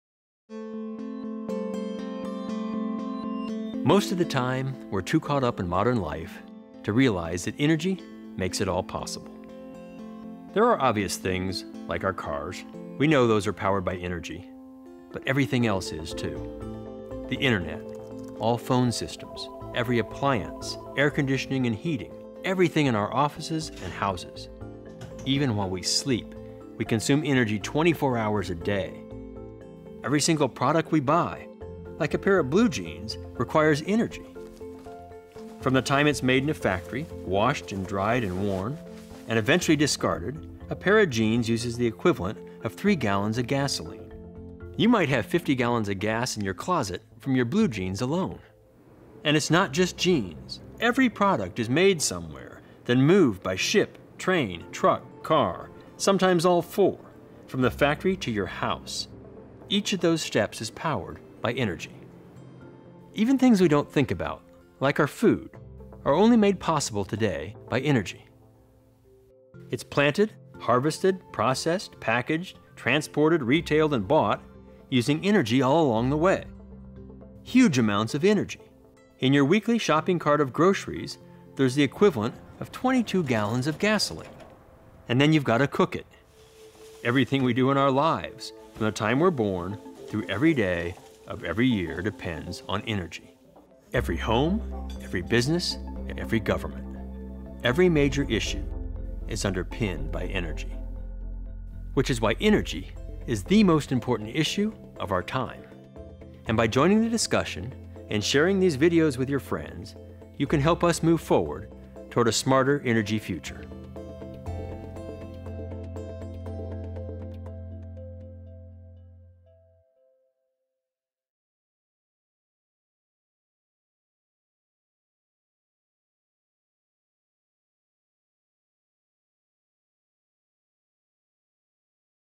Foundation of Modern Life
"From the time it's made in a factory, washed, dried, worn, and eventually discarded, a pair of jeans uses the equivalent of 3 gallons of gasoline." Whether we realize it or not, every aspect of our lives is dependent upon the availability of energy.